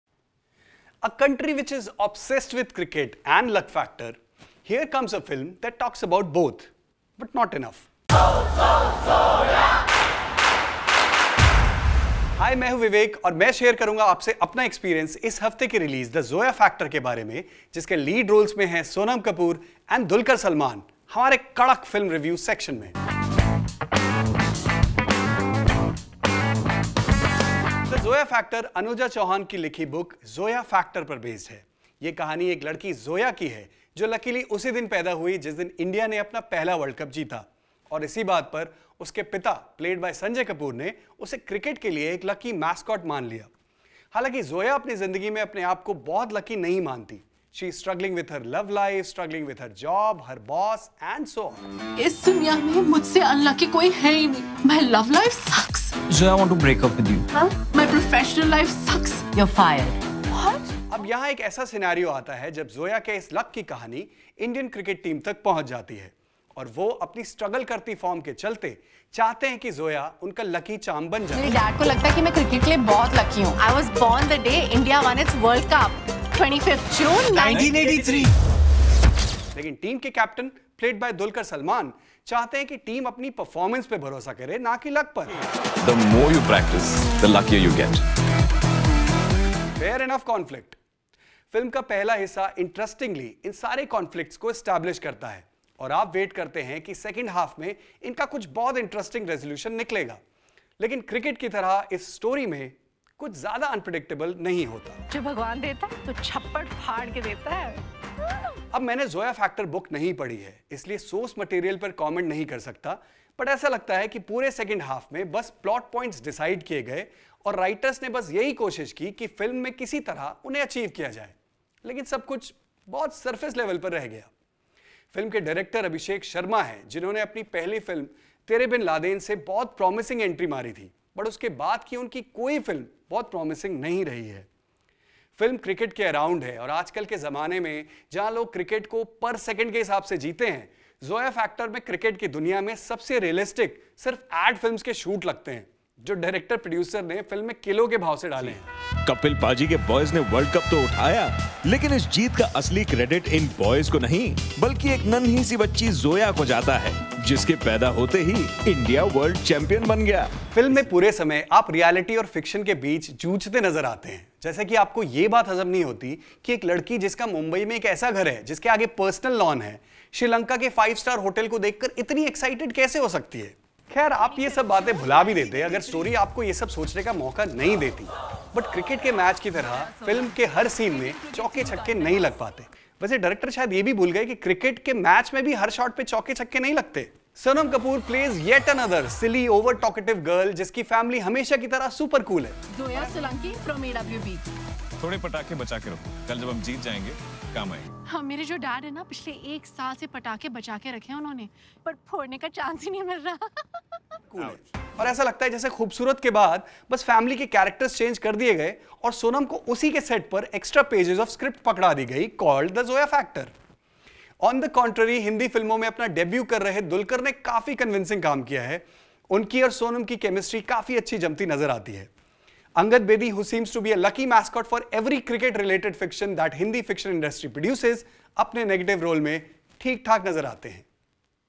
The Zoya Factor movie review by Vivek Jaiswal | Starring Dulquer Salmaan and Sonam Kapoor
SonamKapoor और DulquerSalmaan स्टारर फिल्म TheZoyaFactor आज बड़े पर्दे पर उतर चुकी है। 'द जोया फैक्टर' अनुजा चौहान की इसी नाम की किताब पर आधारित है। जोया सबके लिए लकी है, लेकिन खुद के लिए उसका लक काम नहीं करता है। बस इसी के इर्द-गिर्द फिल्म की कहानी है।

Film Review by Vivek Jaiswal - The Zoya Factor

Cast - Sonam Kapoor, Dulquer Salmaan, Angad Bedi, Sikander Kher, Sanjay Kapoor, Saurabh Shukla, Rahul Khanna
Director - Abhishek Sharma
Production company - Fox Star Studios, Ad-Labs Films Limited
Distributed by - Fox Star Studios